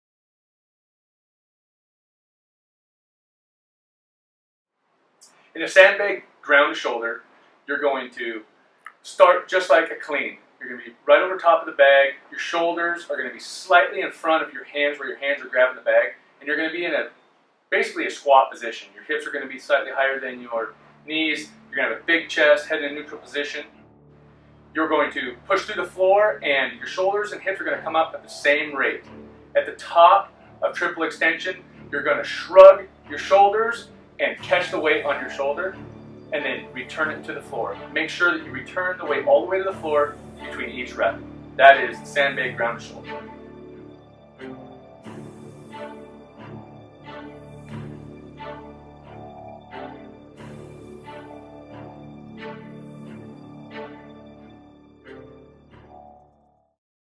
Sandbag Ground to Shoulder
TTH Owner and Trainer Kenton Clairmont demonstrates Sandbag Ground to Shoulder